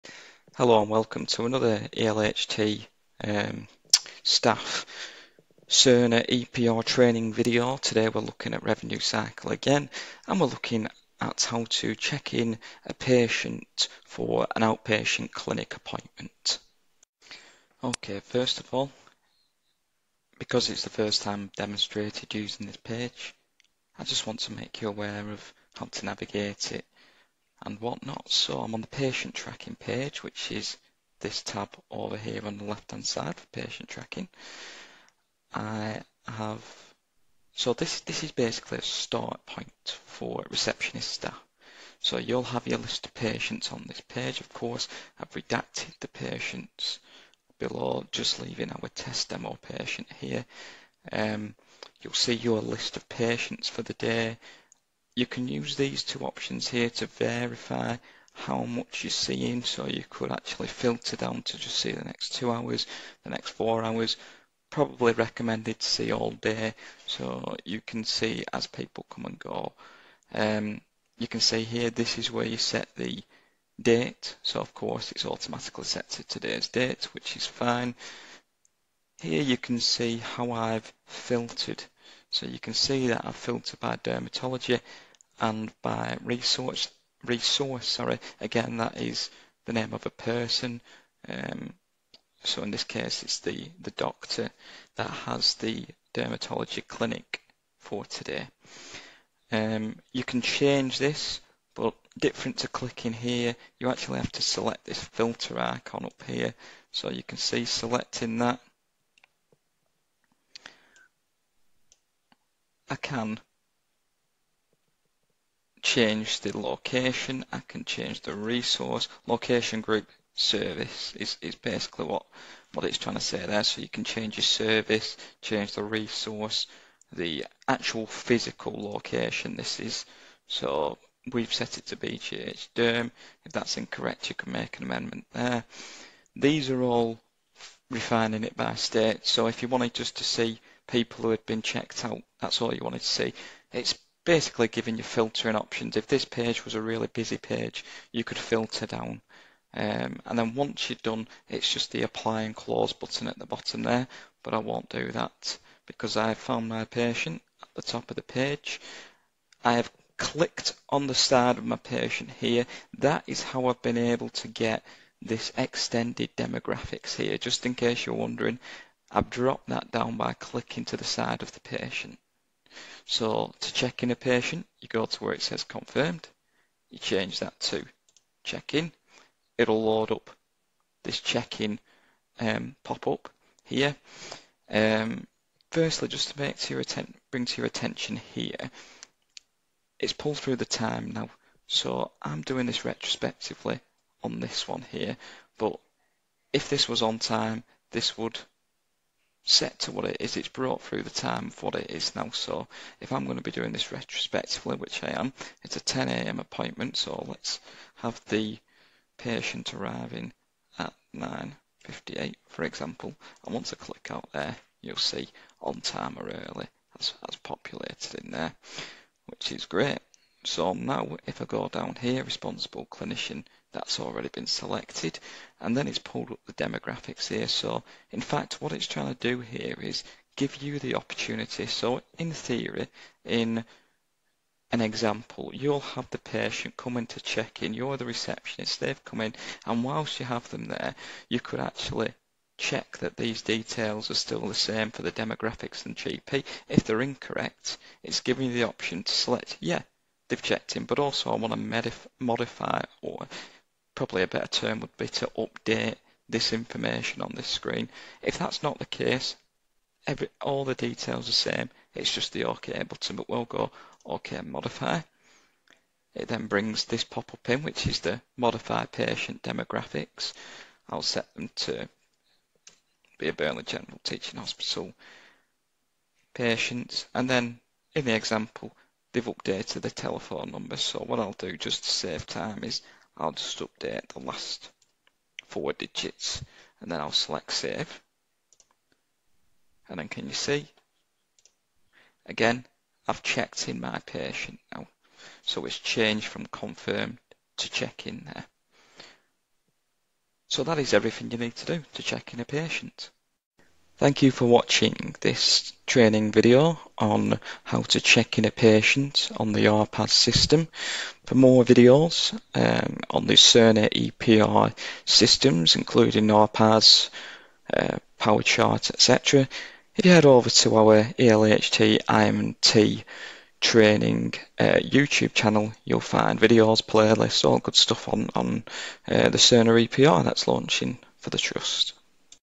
Revenue Cycle (RPAS) OP Clinic – Checking In a Patient
This video is intended for colleagues at East Lancs NHS Trust (ELHT). In this video we will look at how to Check In a patient for an OP Clinic.